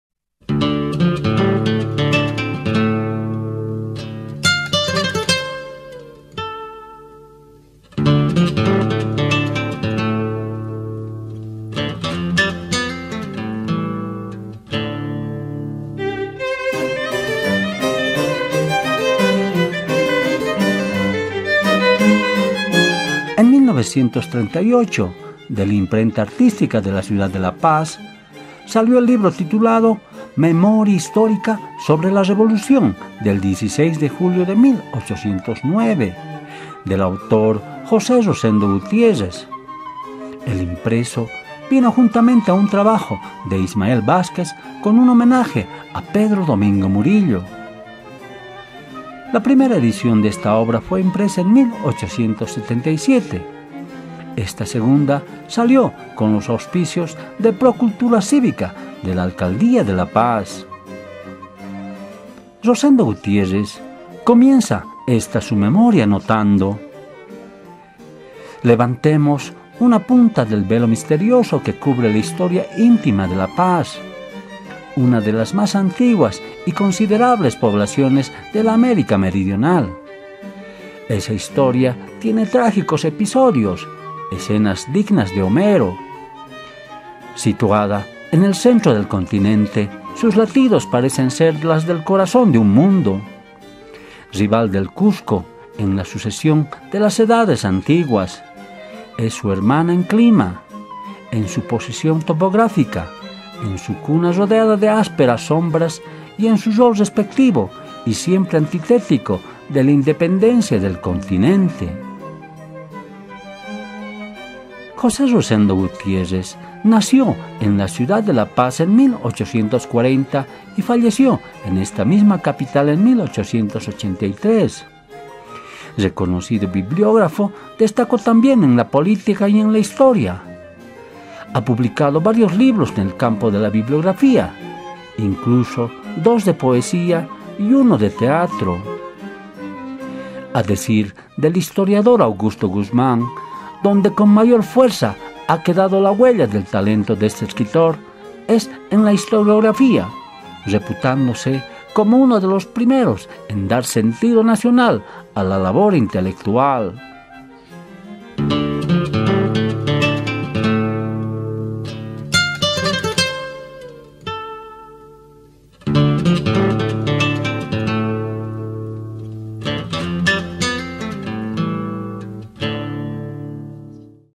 MEMORIA HISTÓRICA SOBRE LA REVOLUCIÓN (1877) de Rosendo Gutiérrez
Bibliógrafo, historiador y poeta. José Rosendo Gutiérrez (La Paz, Bolivia, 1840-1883). Producción del Museo del Aparapita.